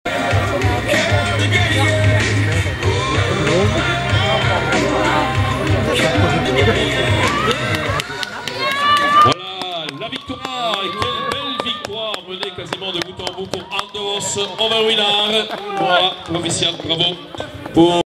2015-01-18 - Ottignies
Overwinning Han Devos in veldrit van Ottignies, Beker van België